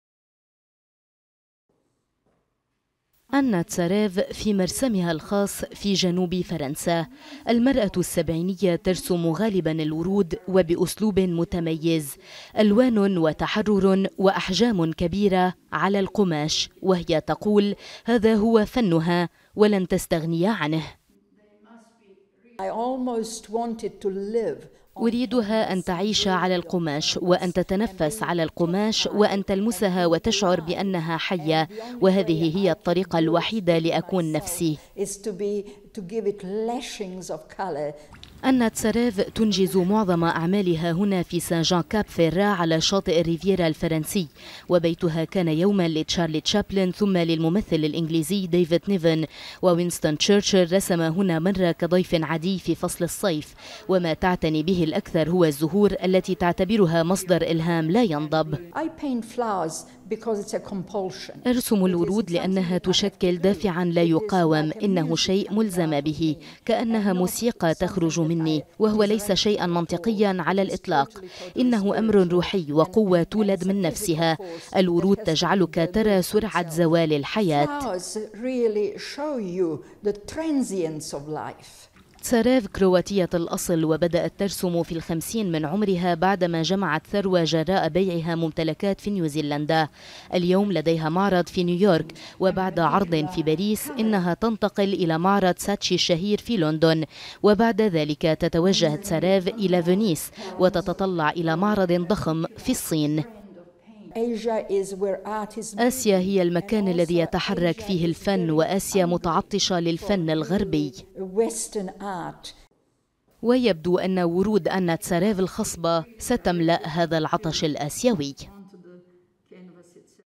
الفنانة آنا تزاريف تحقق نجاحا في عمر الخامسة والسبعين
الفنانة الكرواتية آنا تزاريف، التي أمضت معظم حياتها في نيوزلندا، تحقق نجاحا باهرا وهي في عمر الخامسة والسبعين، من خلال رسم ورود فاتنة على لوحات عملاقة. بعد باريس، ستنتقل أعمالها لتعرض في ساتشي غاليري في لندن هذا الأسبوع. هذا المعرض سيمهد أيضا لآخر في الصين. تلفزيون فرانس برس التقى الفنانة في منزلها الموقت على شاطئ الريفييرا الفرنسي، وهو منزل كن يملكه شارلي شابلن. تقرير مسجل. جورغين هكير/ رندلى جبور